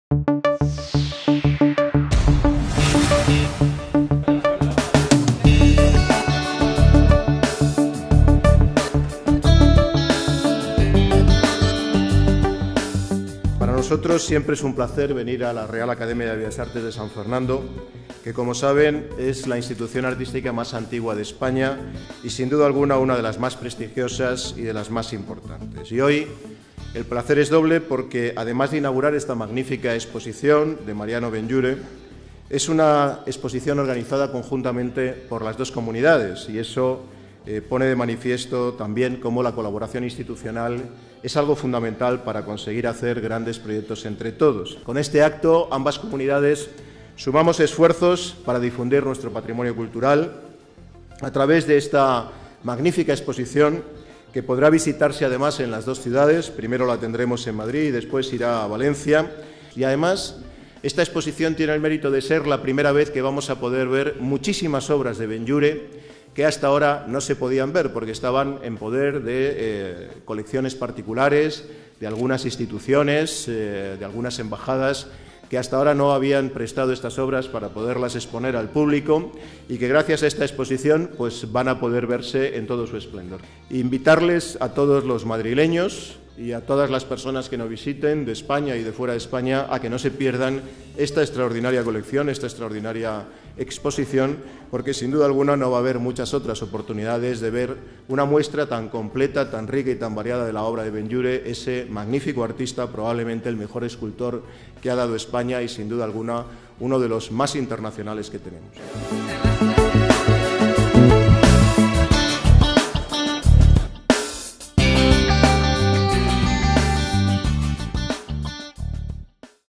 Ignacio González y Alberto Fabra inauguran una exposición de la obra del escultor Mariano Benlliure
El Presidente de la Comunidad de Madrid, Ignacio González, acompañado por el jefe del ejecutivo valenciano, Alberto Fabra, inaugura la exposición Mariano Benlliure. El dominio de la materia, organizada por el Gobierno madrileño y el Consorcio de Museos de la Comunidad Valenciana, con la colaboración de la Real Academia de Bellas Artes de San Fernando.